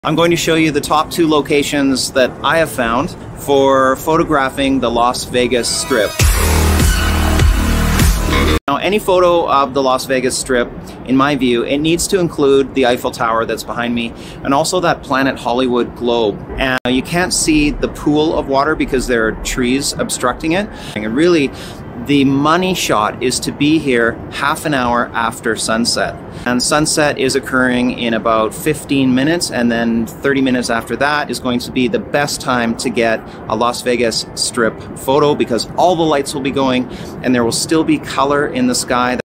Uncovering the Best Secret Locations for Las Vegas Strip Photos
This is the best location in Las Vegas to take cityscape photos.  Watch the entire video here on this channel